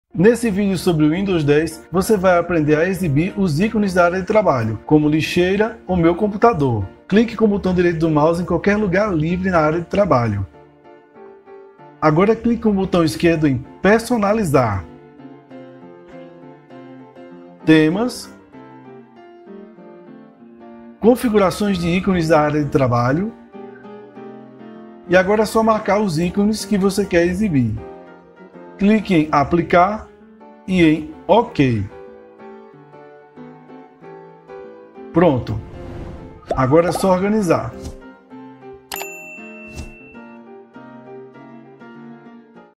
Windows 10 - Adicione ícones da Área de Trabalho
Aprenda a adicionar ícones do Windows 10 na área de trabalho.